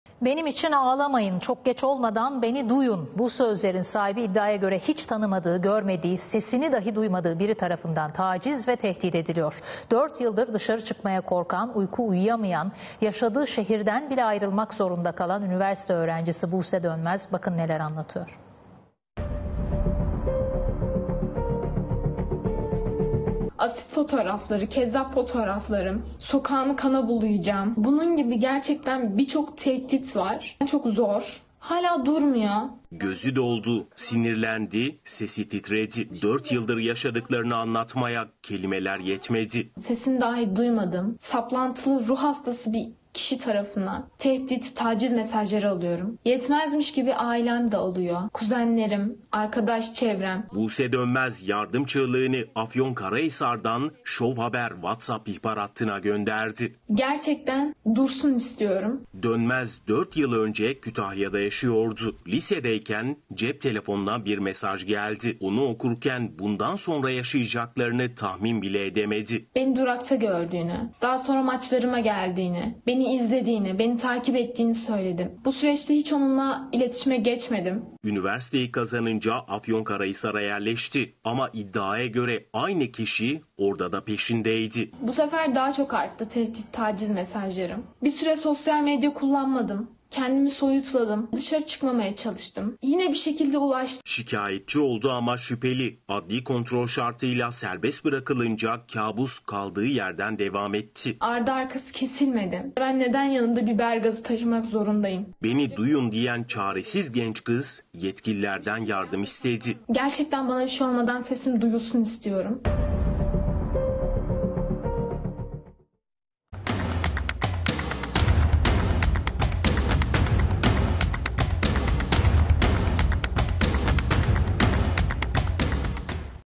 4 yıldır peşinde...
''BENİM İÇİN AĞLAMAYIN, ÇOK GEÇ OLMADAN BENİ DUYUN''... BU SÖZLERİN SAHİBİ İDDİAYA GÖRE HİÇ TANIMADIĞI, GÖRMEDİĞİ, SESİNİ DAHİ DUYMADIĞI BİRİ TARAFINDAN TACİZ VE TEHDİT EDİLİYOR. 4 YILDIR DIŞARI ÇIKMAYA KORKAN, UYKU UYUYAMAYAN,YAŞADIĞI ŞEHİRDEN BİLE AYRILMAK ZORUNDA KALAN ÜNİVERSİTE ÖĞRENCİSİ BUSE DÖNMEZ BAKIN NELER ANLATIYOR?